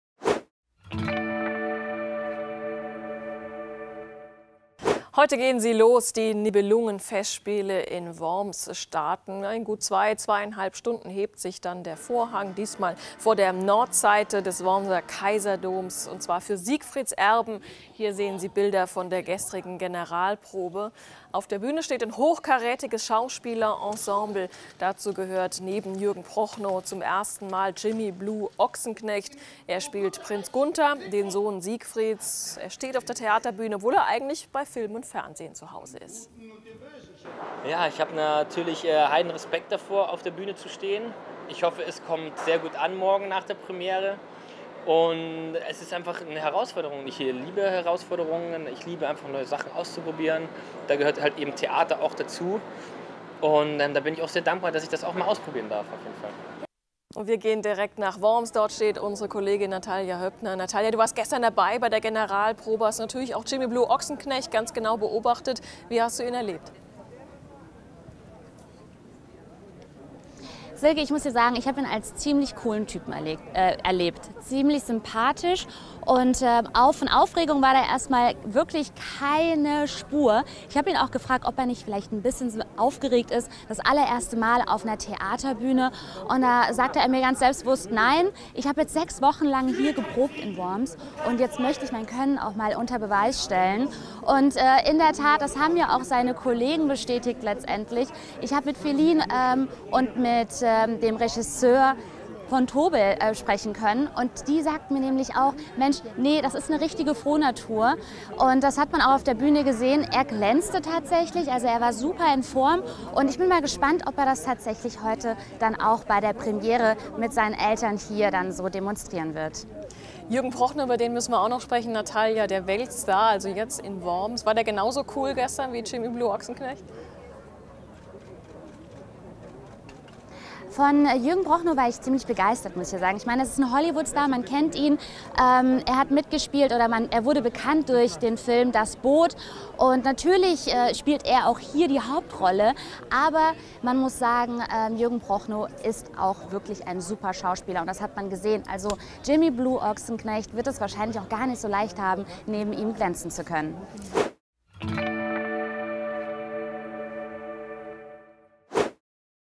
Nibelungenfestspiele in Worms | RON TV
Heute starten die Nibelungenfestspiele in Worms. Unsere Reporterin Natalia Höppner ist vor Ort und hat die Eindrücke vom roten Teppich.